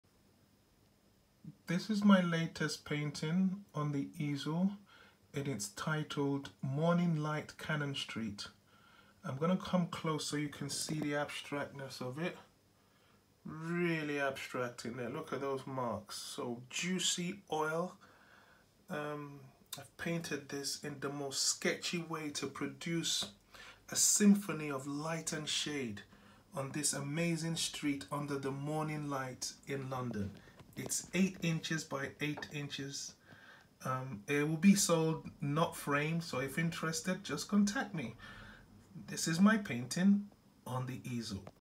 ON THE EASEL 2-MORNING LIGHT, CANNON STREET.
This is a little painting I’ve just completed and I did it to show the beauty of light and shade in this street in London in glorious mornings. It’s 8” x 8”.